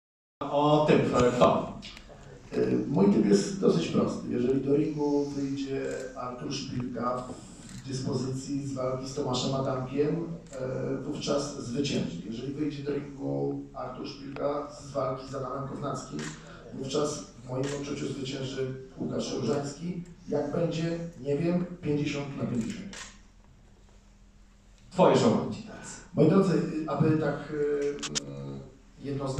MARCIN NAJMAN TYPUJE SZPILKA VS RÓŻAŃSKI
Taki smaczek z konferencji prasowej KBN 15 czyli typ Marcina Najmana na walkę z Artura Szpilki z Łukaszem Różańskim. Jeśli nie wiecie na kogo postawić to ten materiał może ułatwić zadanie!